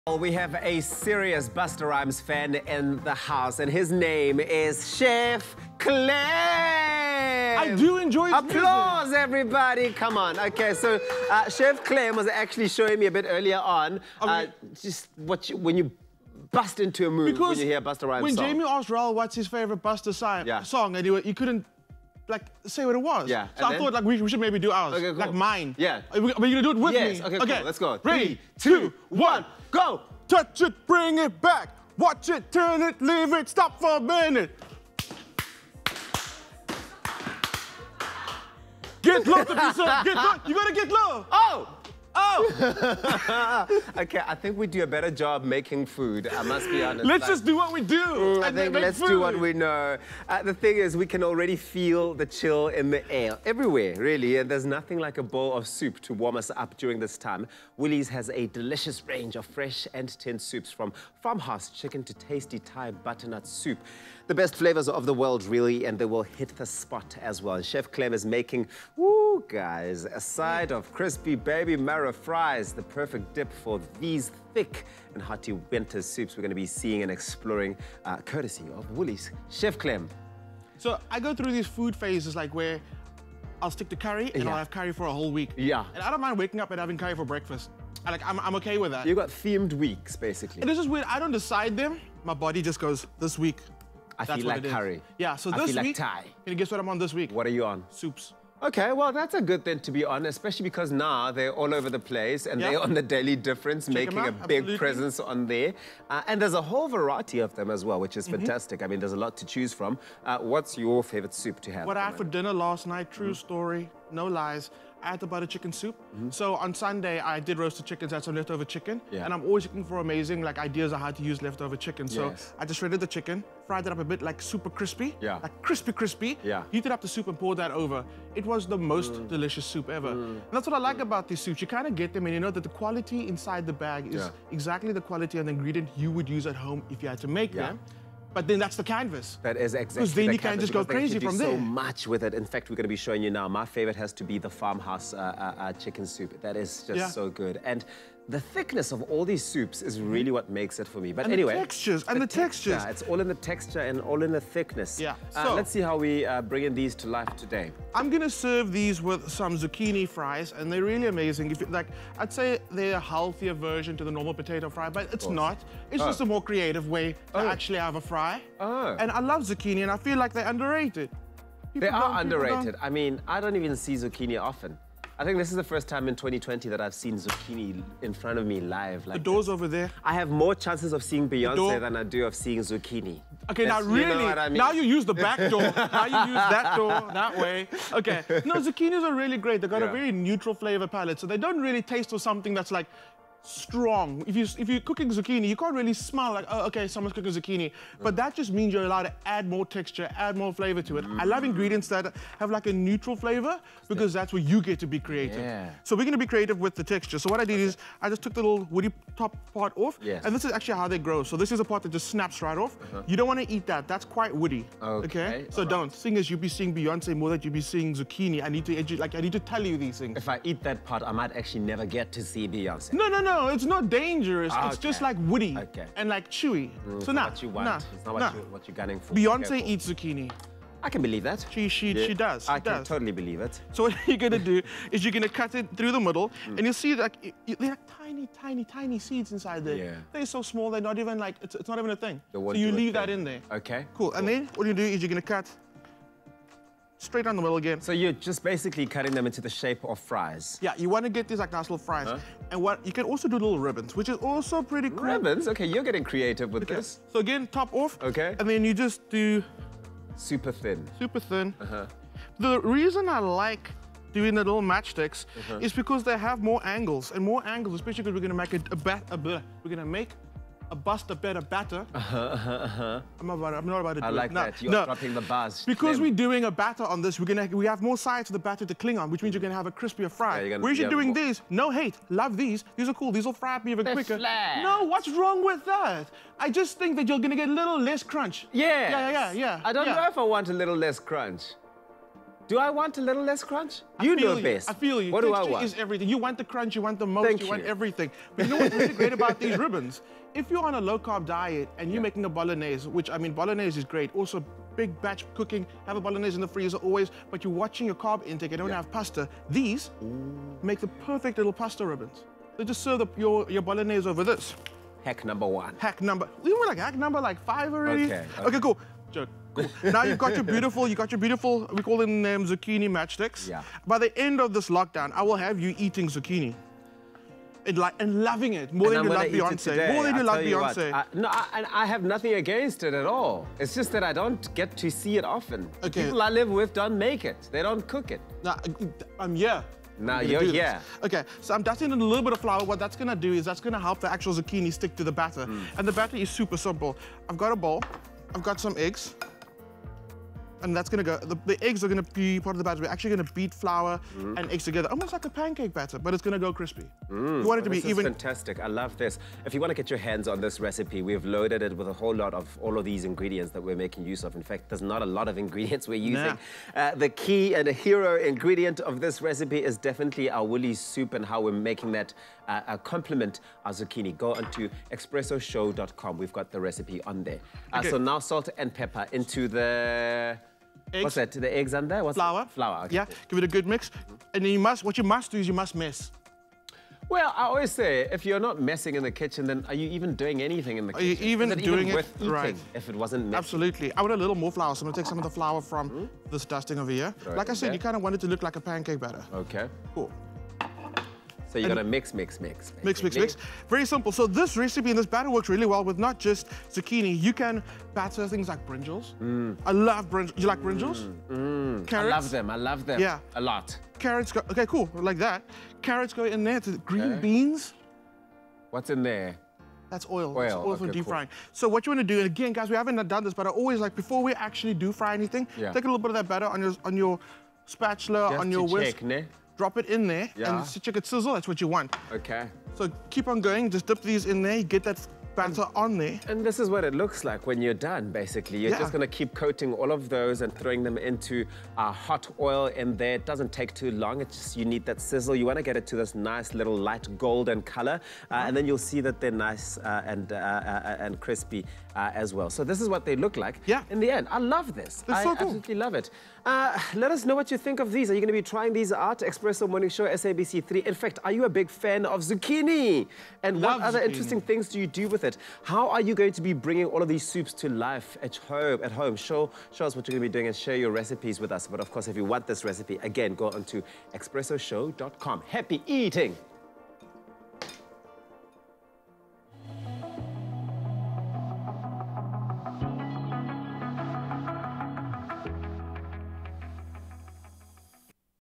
Recipe: Crispy Baby Marrow Fries (WW)
Chef Clem is going to show us how to make crispy baby marrow fries to serve with your soup.